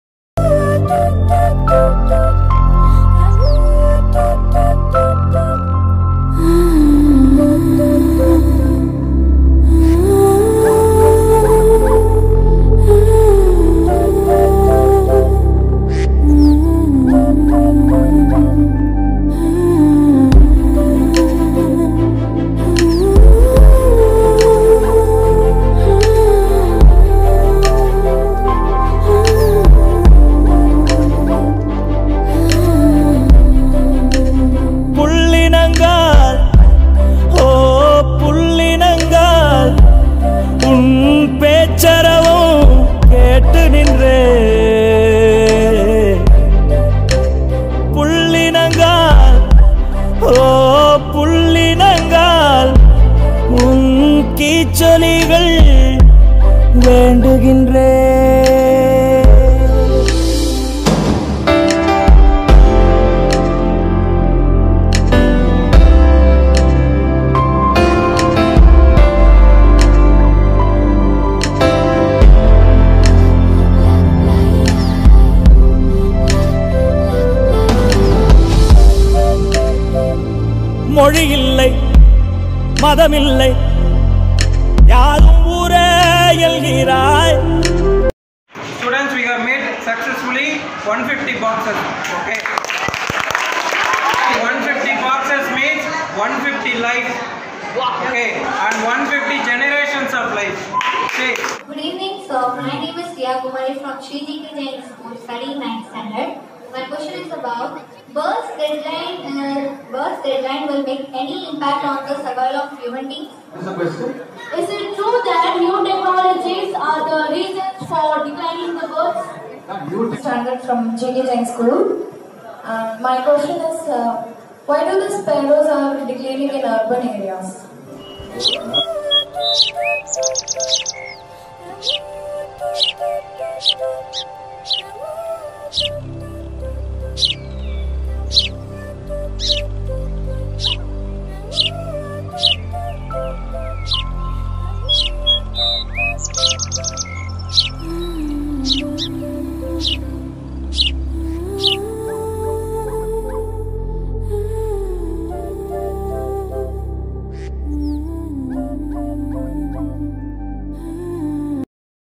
Sparrow Nests Building Project by our students in association with NGo KOODUGAL
Video from @GKJAINSCHOOLS